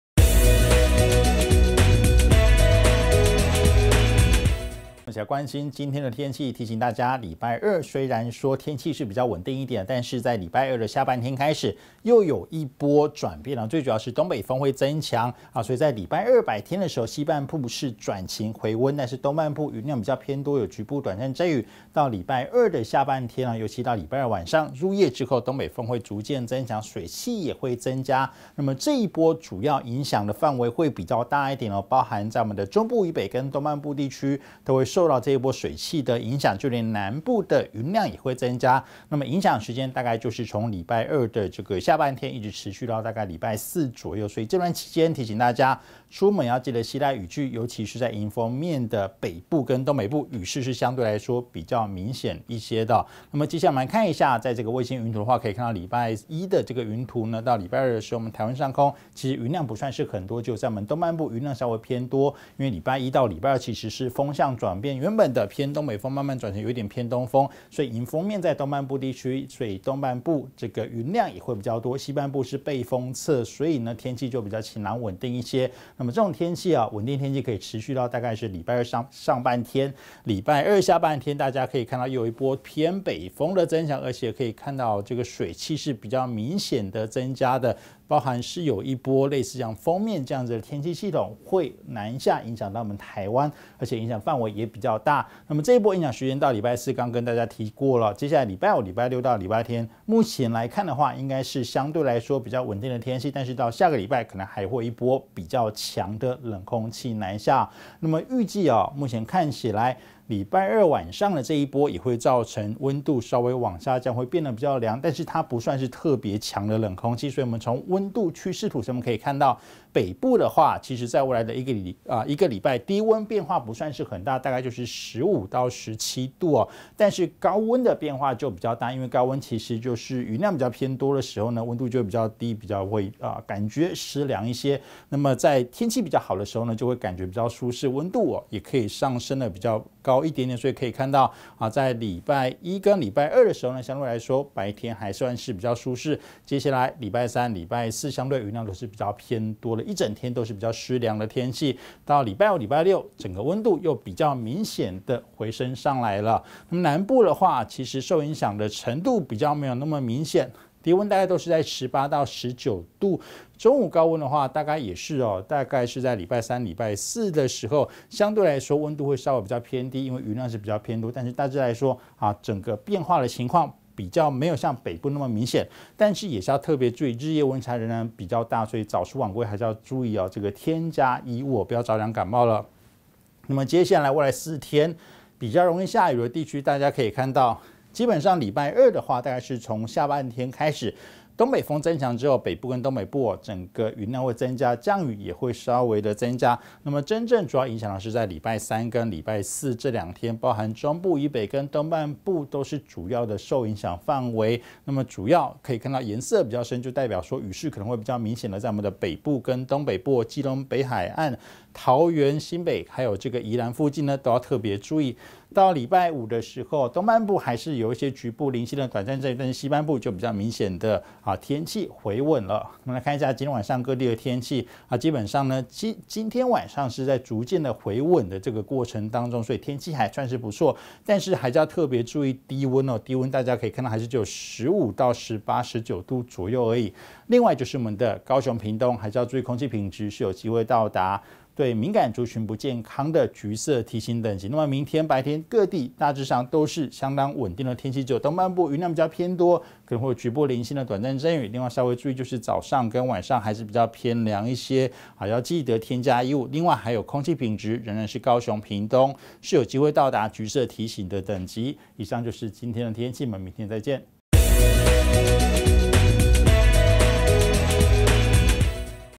2019/01/14 西半部轉晴回溫 東半部雲多偶雨
西半部轉晴回溫
東半部雲多偶雨
周二晚起東北風增強水氣增
周三、四中部以北濕涼有雨